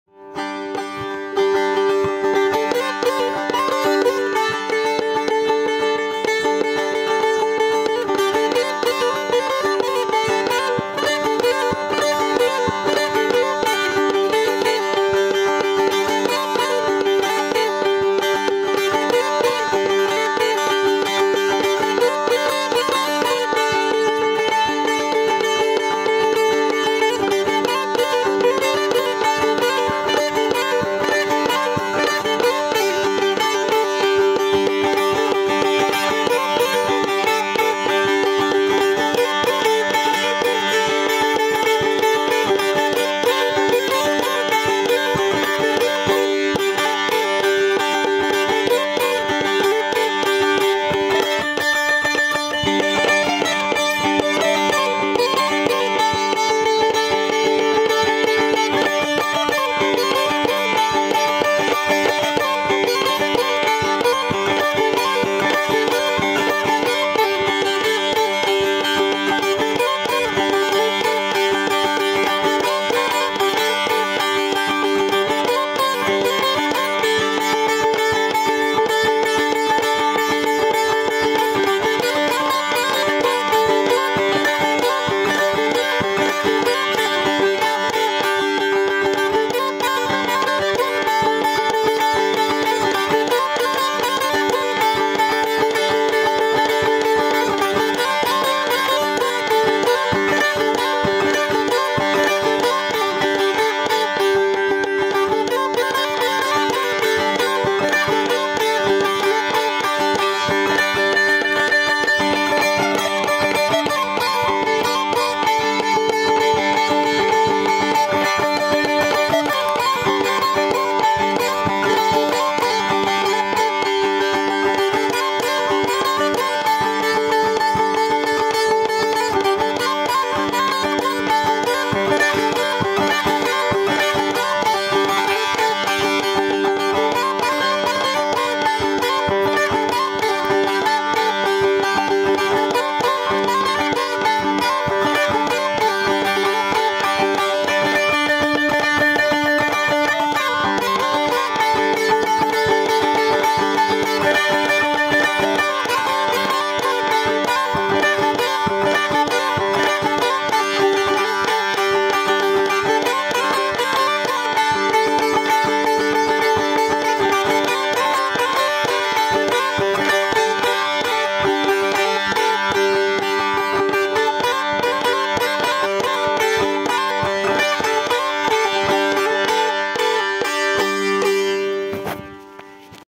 Chitrali Sitar Playing by Young Artist Saqib Ali From Ghizer
Chitrali Sitar
By Saqib Ali